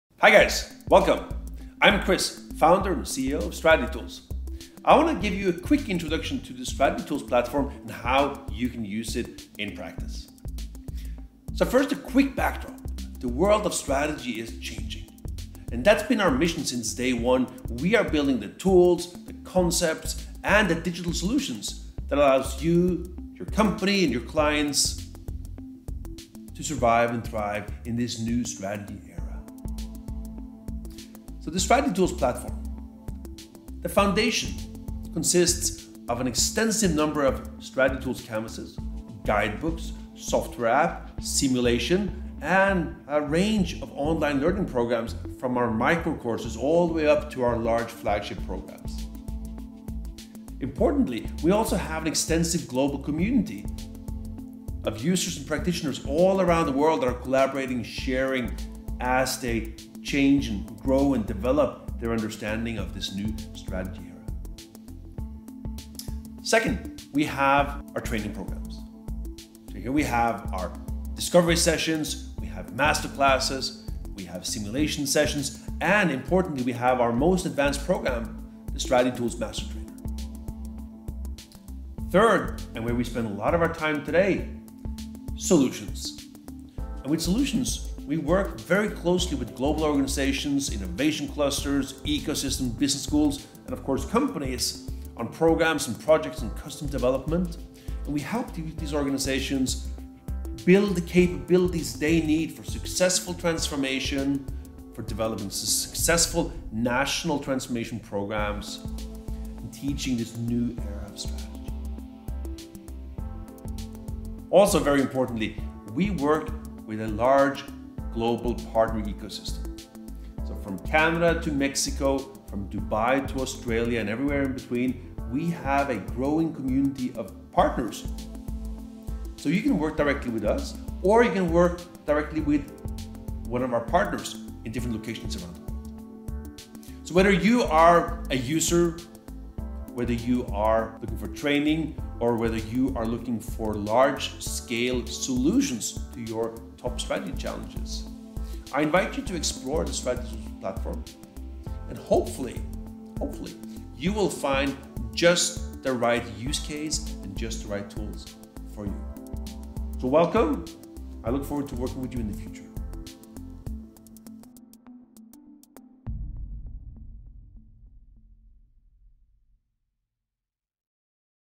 Introduction to the Strategy Tools Platform
Strategy Tools Founder & CEO Christian Rangen gives a quick tour of the Strategy Tools Platform and what you can do with it.
From strategy canvases, digital learning programs, strategy software to large-scale custom-built solutions, the Strategy Tools Platform looks to change how the world works on strategy.